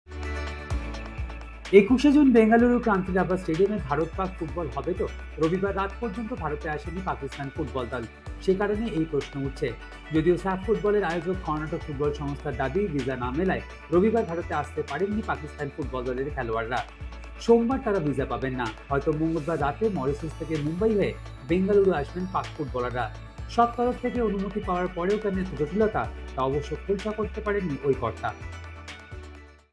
SAFF Footbal 2023 : ভিসা জটে পাক ফুটবল দল, কবে আসবে ভারতে ?
২১ জুন বেঙ্গালুরুর ক্রান্তিরাভা স্টেডিয়ামে ভারত-পাক ফুটবল হবে তো ? রবিবার রাত পর্যন্ত ভারতে আসেনি পাকিস্তান ফুটবল দল।